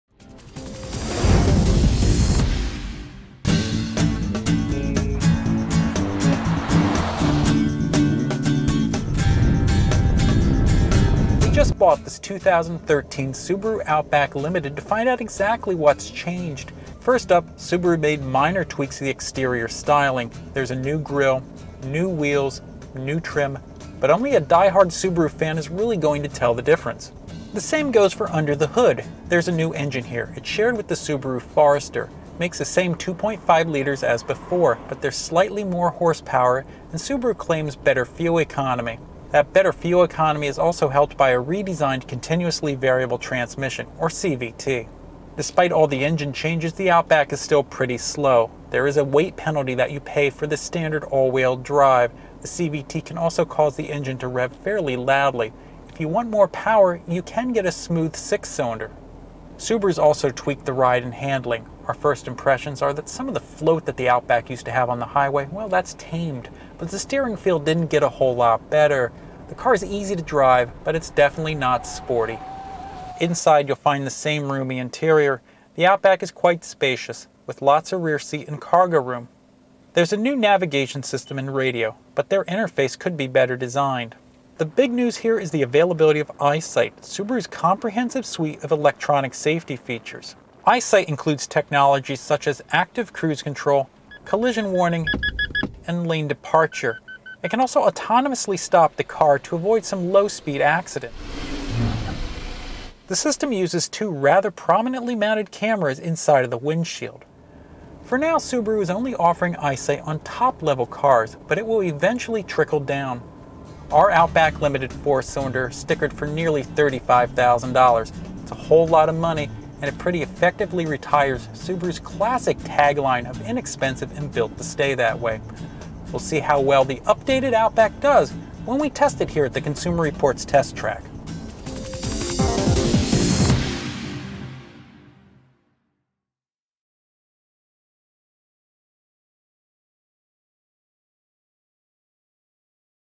2013 Subaru Outback first drive | Consumer Reports
Updated for 2013, the popular Subaru Outback has a new engine, new transmission, and tweaks to the ride and handling. Subaru's new EyeSight system adds many electronic safety technologies.